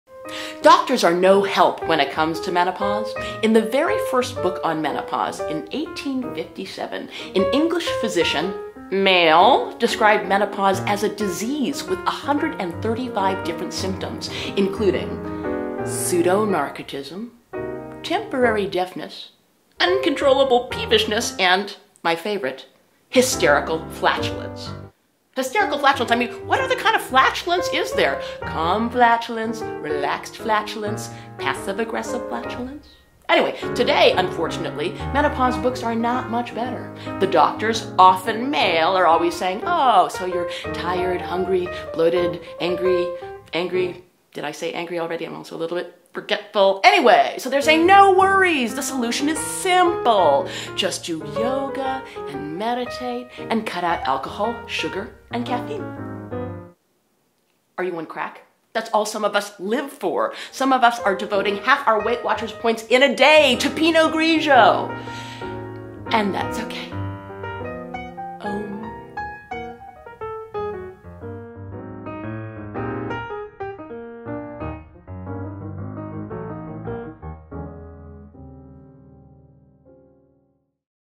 Sandra Tsing Loh: The B***h is Back
Welcome to Sandra Tsing Loh's menopause! It's a beautiful thing. Here is another little sneak peek of her upcoming comedy show, The B is Back, An All-Too Intimate Evening at The Broad Stage, July 9-August 2, 2015.